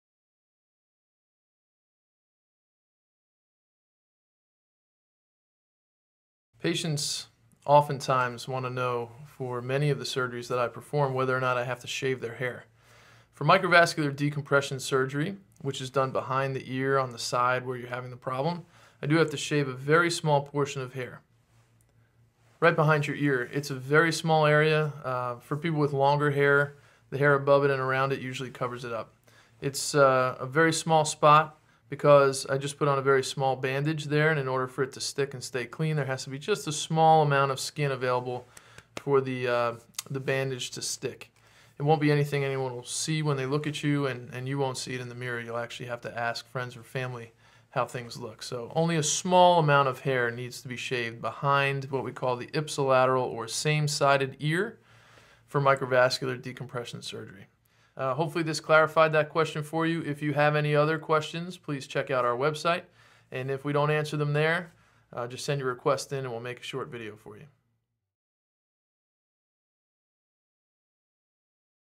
Neurosurgeons of New Jersey : Is My Head Shaved During Microvascular Decompression Surgery?
Many of our patients ask if they are required to shave their head prior to microvascular decompression surgery. Dr. Anthony D'Ambrosio explains.